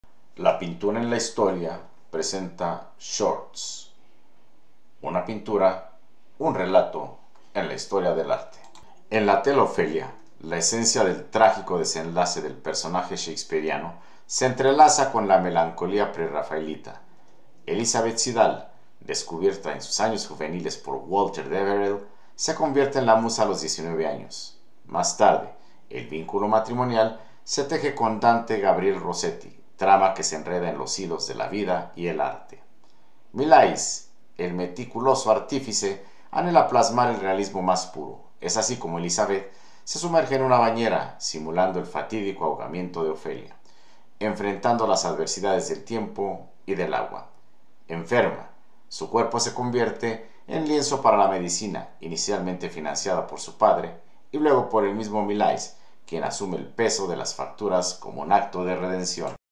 Ofelia, John Everett Millais, cápsula 1
Con apenas 19 años, Elizabeth se convierte en el pincel viviente para dar vida al desgarrador destino de Ofelia, cuyo suspiro se ahoga en las aguas turbias del arroyo. En la escena cargada de realismo, Millais, el artista meticuloso, persigue cada matiz de la tragedia mientras Elizabeth se sumerge en una bañera, su cuerpo acariciado por las aguas que emulan la implacable corriente de la muerte.